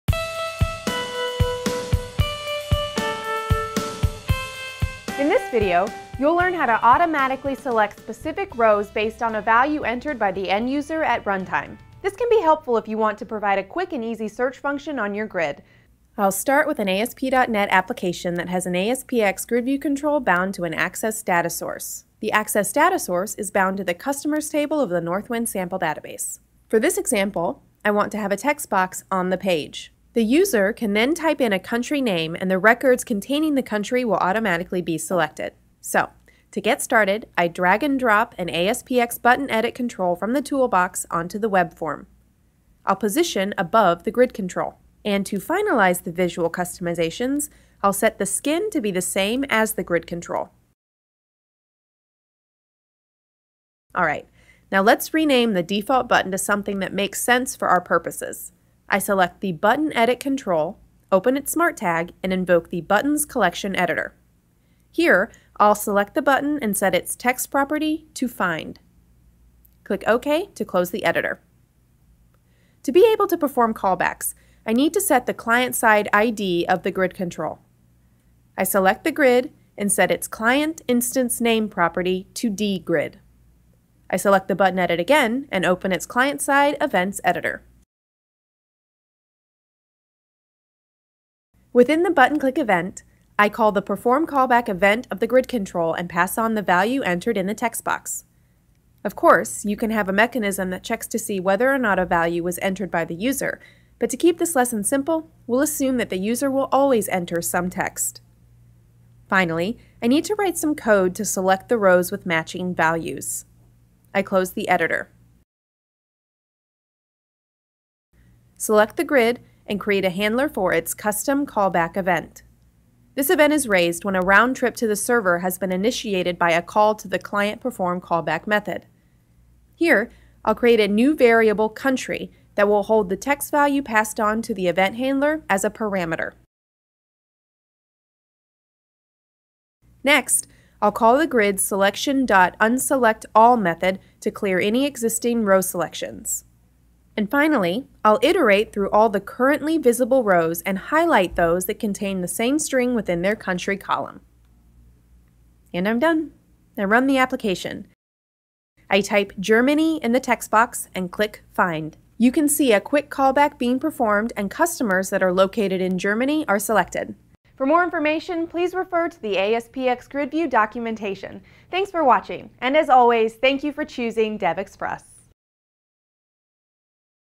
ASP.NET Grid - How to Select Rows That Contain the Specified Value
This video covers our 2008 release.  The ASPxGridView is DevExpress' Grid Control for ASP.NET.  In this video, you will learn how to automatically select specific rows based on a value entered by the end-user at runtime. This can be helpful if you want to provide a quick and easy search function on your grid.  For more information on our subscriptions, please visit...